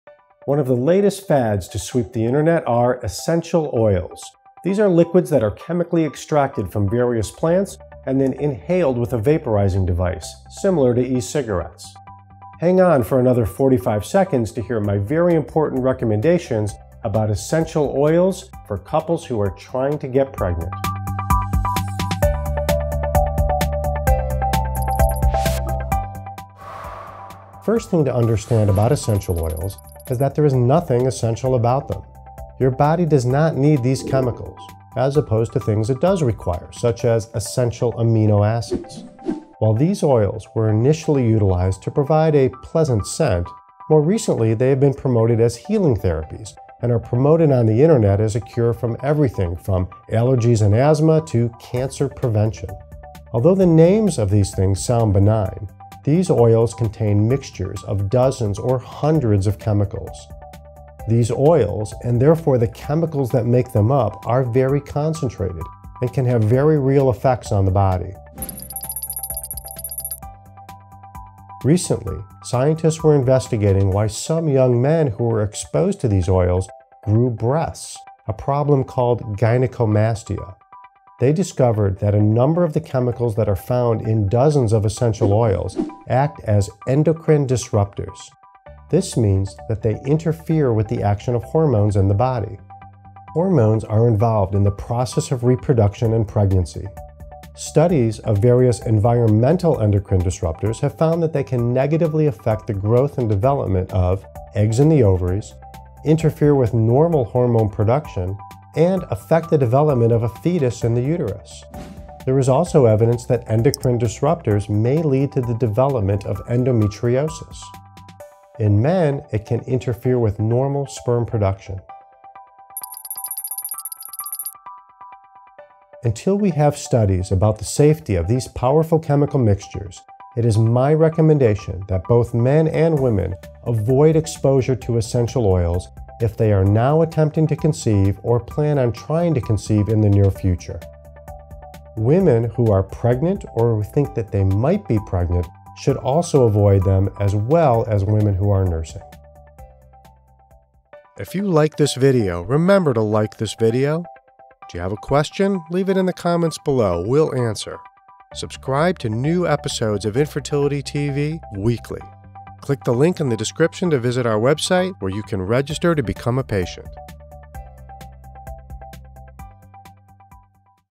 Essential Oils - Should you avoid them while trying to conceive - TTC ?
Should essential oils be avoided when you are TTC? Some essential oils are endocrine disrupters that may affect normal development or cause endometriosis. Listen in as Dr Randy Morris MD-The BOARD CERTIFIED fertility expert explains on InfertilityTV

One of the latest fads to sweep the internet are essential oils. If you are not familiar, these are liquids that are chemically extracted from various plants and then released into the air through a vaporizing device and then inhaled into the body. In this respect, they are similar to E-cigarettes.

Hang on for another 45 seconds to hear my very important recommendations about essential oils for couples trying to get pregnant.

The first thing to understand about essential oils is that there is nothing essential about them. Your body does not need these chemicals as opposed to things it does require such as essential amino acids.

While these oils were initially utilized to provide a pleasant scent, more recently they have been promoted as healing therapies and are promoted on the internet as a cure all for everything from allergies and asthma to cancer prevention.

Although the names sound benign, these oils contain mixtures of dozen or hundreds of chemicals. These oils, and therefore the chemicals that make them up, are very concentrated and can have very real effects on the body.

Recently, scientists were investigating why some young men who were exposed to these oils grew breasts, a problem called gynecomastia.

They discovered that a number of the chemicals that are found in dozens of oils act as endocrine disrupters. This means that they interfere with the action of hormones in the body.

Hormones are involved in the process of reproduction and pregnancy. Studies of various environmental endocrine disrupters have found they can negatively affect the growth and development of eggs in the ovaries, interfere with normal hormone production and affect the development of a fetus in the uterus.
There is evidence that endocrine disrupters may lead to the development of endometriosis.

In men, it can interfere with normal sperm production.

Until we studies about the safety on these powerful chemical mixtures, it is my recommendation that both women and men avoid exposure to essential oils if they are now attempting to conceive or plan on trying to conceive in the near future.

Women who are pregnant or think they might be pregnant should also avoid them as well as women who are nursing.